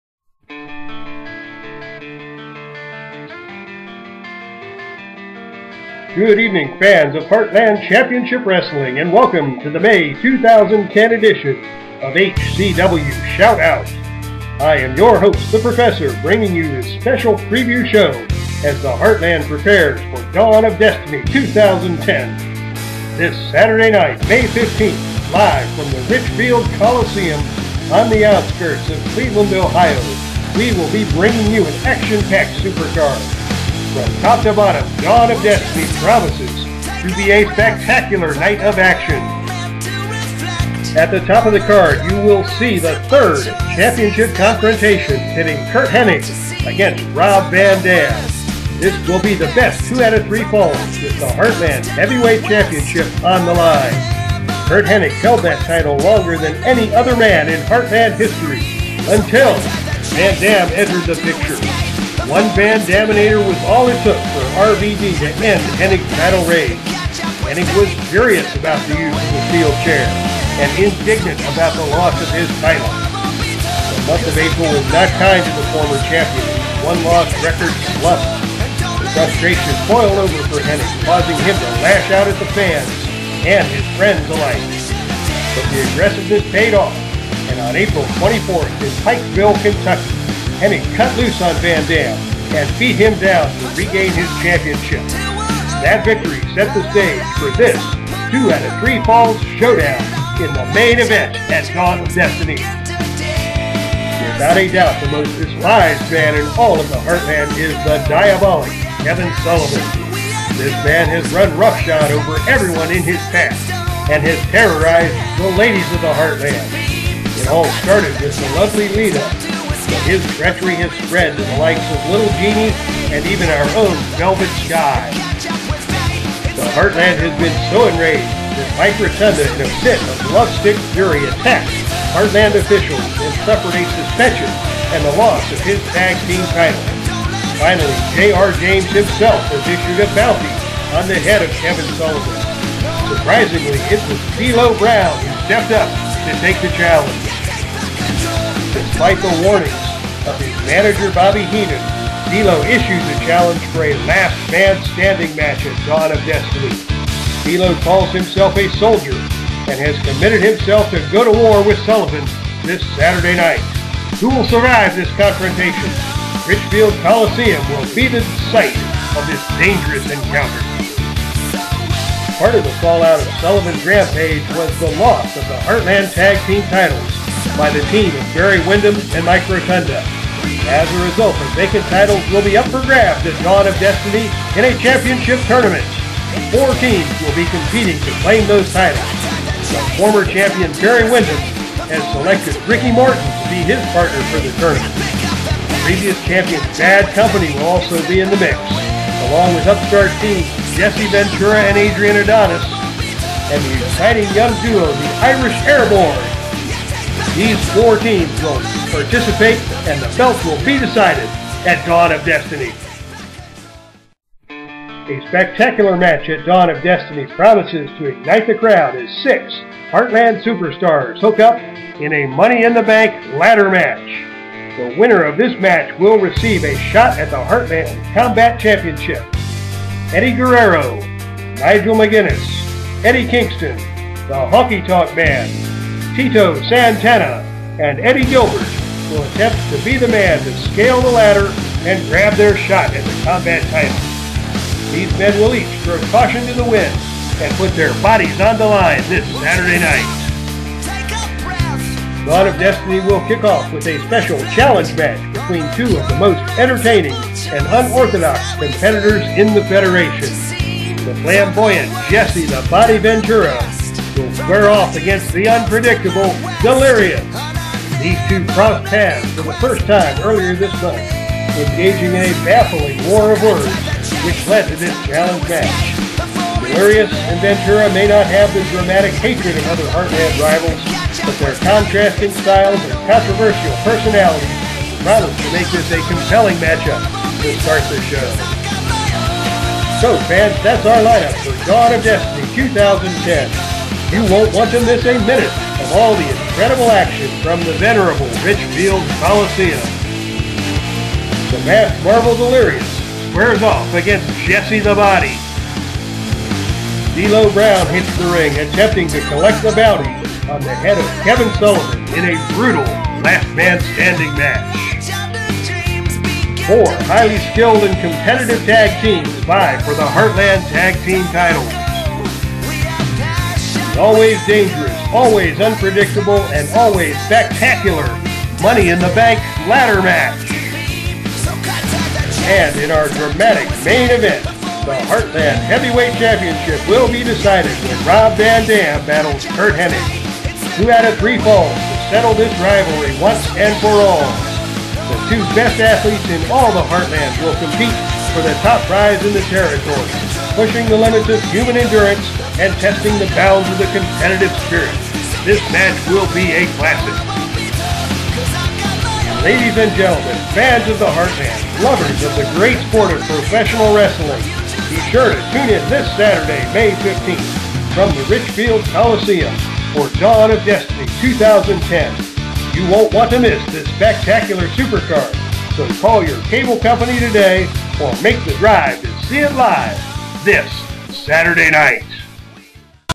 DOD2010.wmv
Promo video for the upcoming Pay-per-view of Heartland Championship Wrestling!